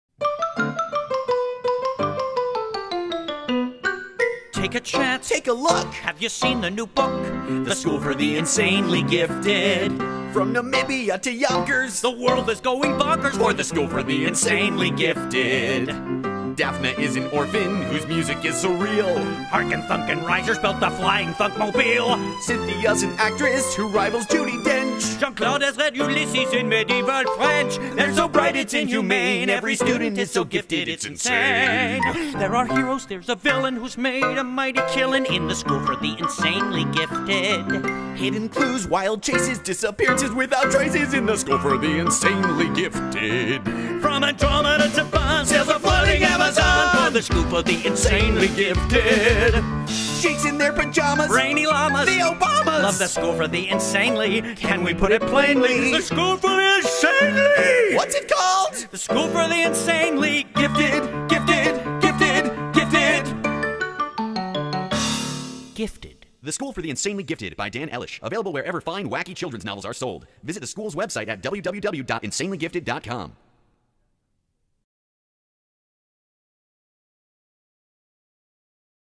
The School for the Insanely Gifted
A song and video about the new comic children's novel, The School for the Insanely Gifted, by Dan Elish, published in June, 2011 by Harper Collins.